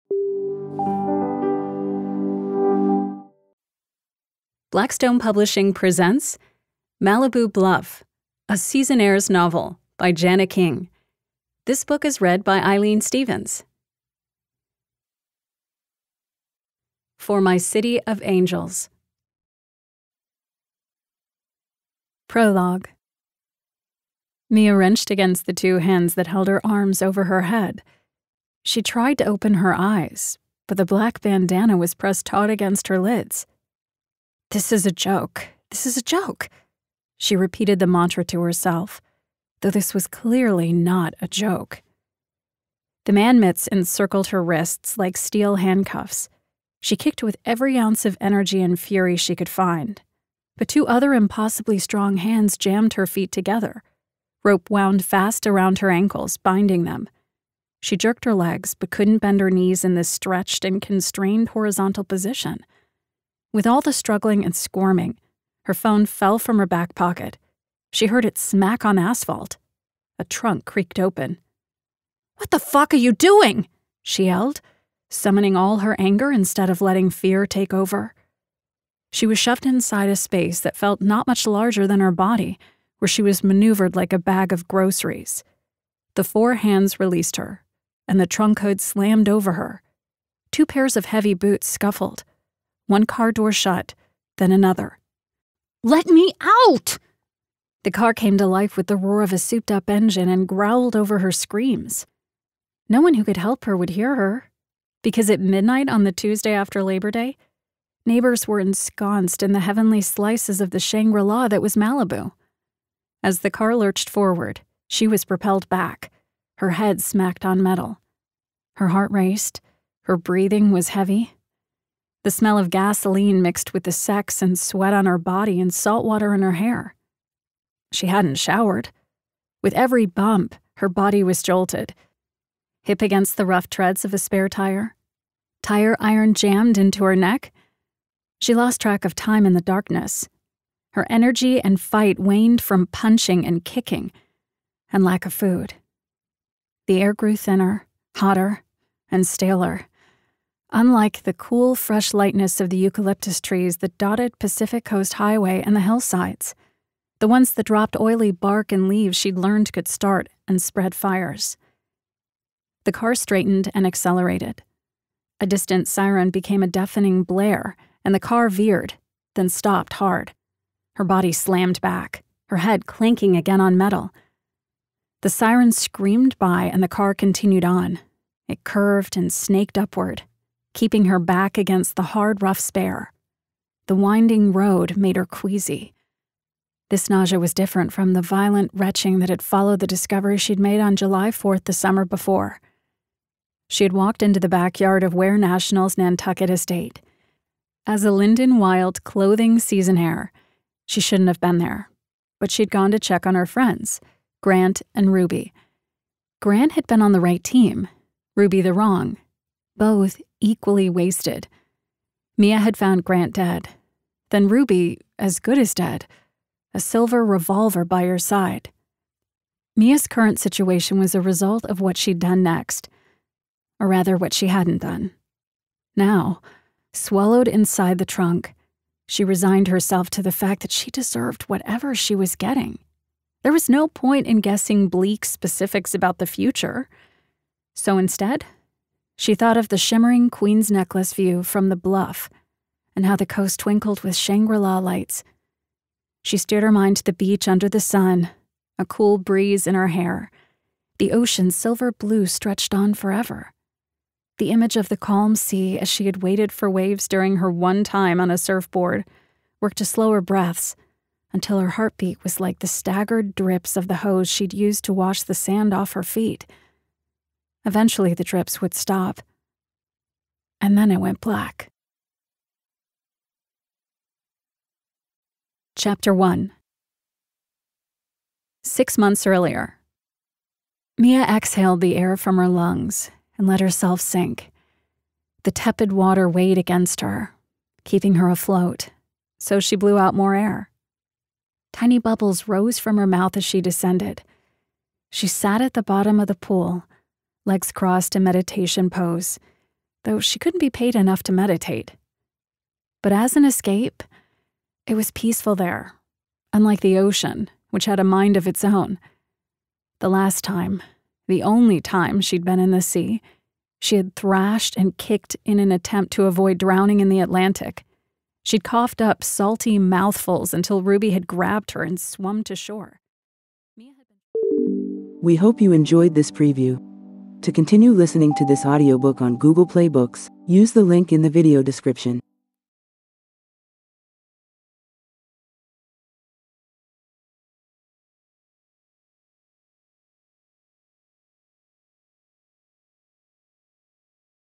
Malibu Bluff: A Seasonaires Novel by Janna King · Audiobook preview
Malibu Bluff: A Seasonaires Novel
Authored by Janna King
Narrated by Eileen Stevens

0:00 Intro
0:03 Malibu Bluff: A Seasonaires Novel
0:24 Prologue
4:35 One
5:24 Outro

BOOK DESCRIPTION

A fresh crew of seasonaires hit the California coast and make Malibu their playground in the sizzling and suspenseful follow–up to The Seasonaires.

Every summer is designed as a dream for six twenty–something seasonaires chosen by Lyndon Wyld, the founder of her namesake clothing line. This summer takes these influential brand ambassadors to the West Coast, led by last season’s Mia, who has been roped back in after her mother’s death by a sweeter deal and the hope to leave her grief behind for the California sun.

Mia is thankful that she won’t have to live with another former seasonaire, Presley, who is now handling Lyndon Wyld’s public relations in Los Angeles after making a meal out of being falsely accused of murder. In Malibu, Mia will share a stunning modern manse with Eve, an outspoken activist; Alex, a gorgeous boundary pusher; Chase, a professional surfer; Oliver, a preppy charmer; and Brandon, the son of Lyndon’s business partner, and the young producer of the brand’s new digital channel, which will up the seasonaires’ social media game.

Lyndon has doubled the salary for her flock to loll on Malibu’s beaches, hobnob at Hollywood parties, and live the “planned casual” SoCal lifestyle. Their antics are juicy entertainment for their throngs of fans and followers. But detractors from Mia’s past come back to haunt her. And when the line between what’s real and what’s staged gets blurred, the results could be deadly.

Janna King is a screenwriter, playwright, and director. She has written television movies, television series, and two short films. She lives in Los Angeles.

Eileen Stevens is a voice–over actress whose voice can be heard on cartoons, promos, programs for English–language learners, and audiobooks. An Earphones Award–winning narrator, she is also an audiobook director and producer.

AUDIOBOOK DETAILS

Language: English
Publisher: Blackstone Publishing
Published on: May 7, 2019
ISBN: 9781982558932
Duration: 9 hr, 45 min
Genres: Fiction / Crime, Fiction / Women